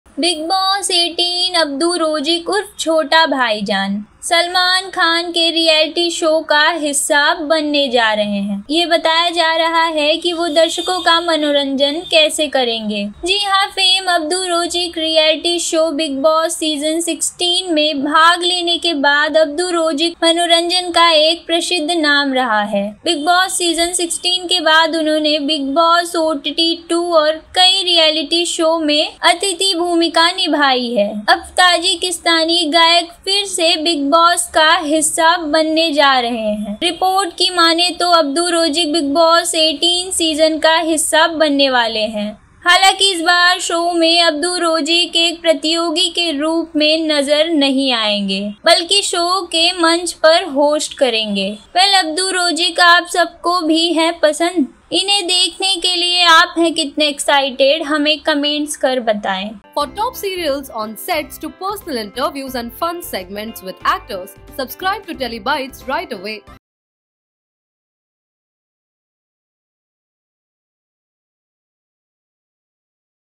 Bigg Boss 18: Abdu Rozik Ki Entry Hui CONFIRMED; Abdu Laenge Salman Ke Saath Milkar TWIST
Voice Over- Anjali Dubey